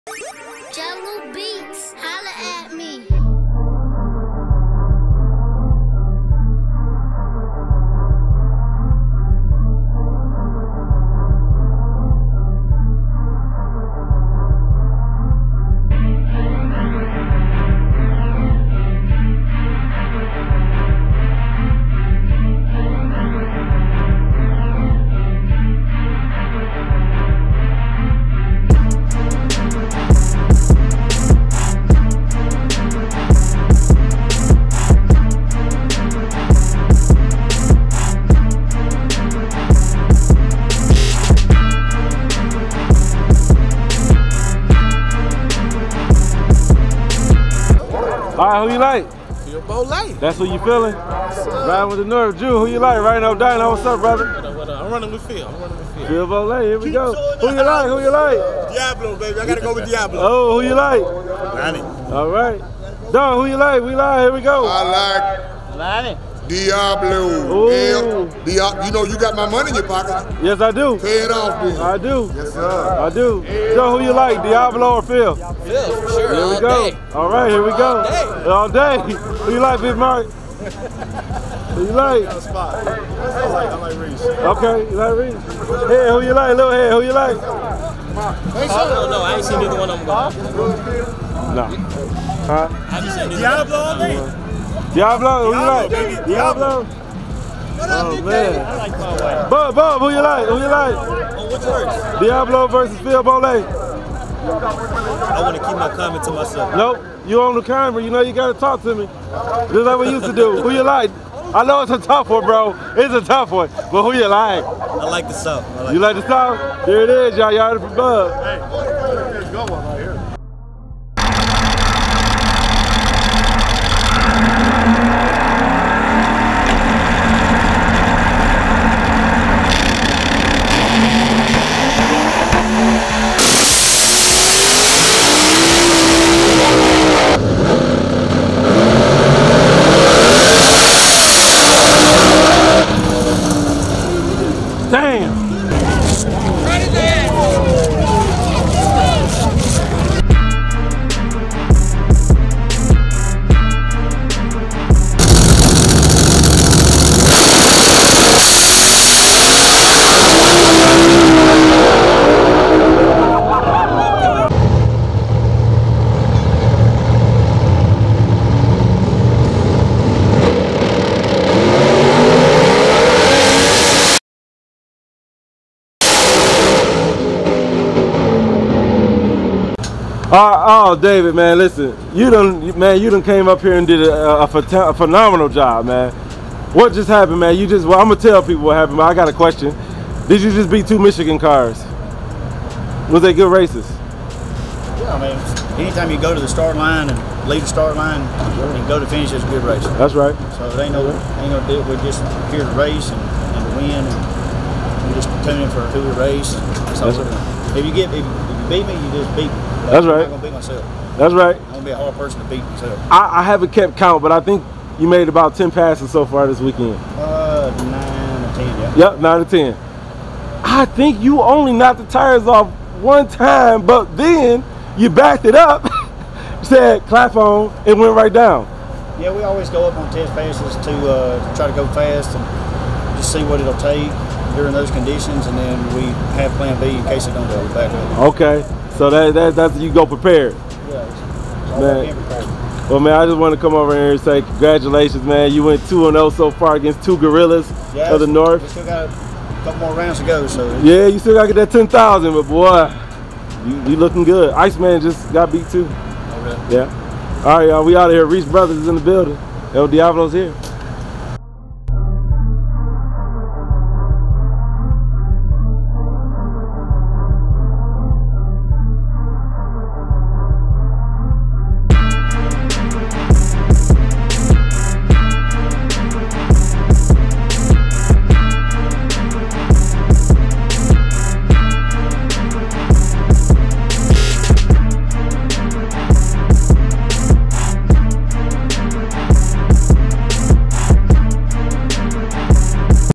David "EL-DIABLO" Reese came to the NORTH with a purpose! - Payso Productions
David-mandi Reese / Reese Brothers Racecars Shop came to the MIGHTY North prepared to do whatever it takes to cross the finish line first. They overnighted parts to the track, had a parts failure and still continue to dominate in this tough field of 28" Extreme N/T

Dave we appreciate you making the long trip and you sir have a baaaaddddd mamaaa jamaa! OHHHHHHWEEEEEEEEEEE!